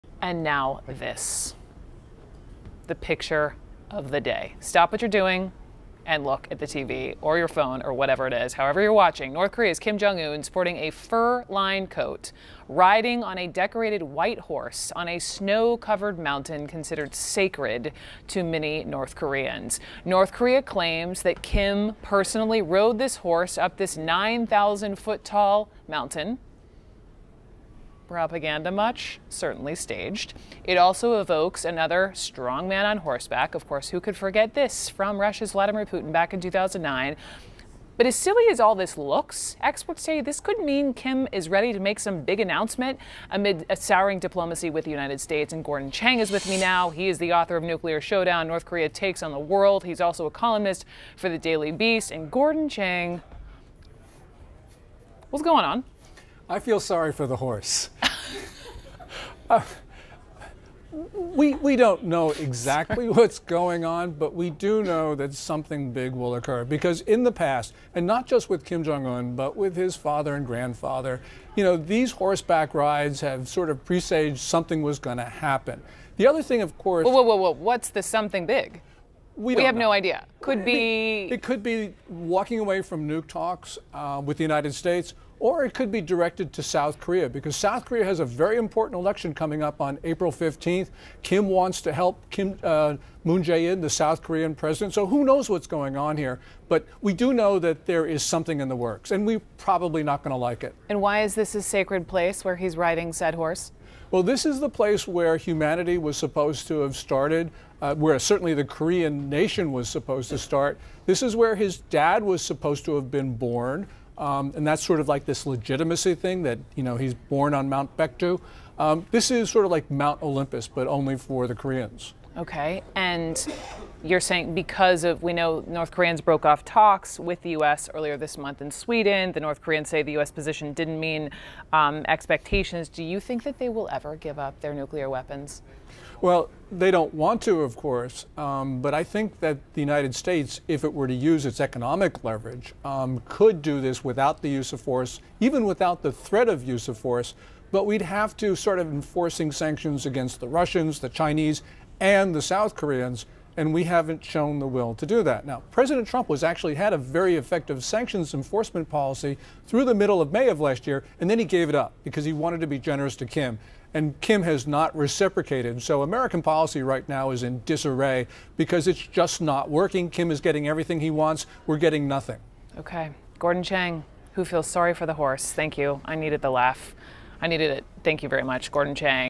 Expert: Kim Jong Un photo shows something big about to happen
North Korean state media released pictures of Kim Jong Un riding a white horse up a mountain in what is considered a politically significant act. CNN's Brooke Baldwin discusses the possible ramifications.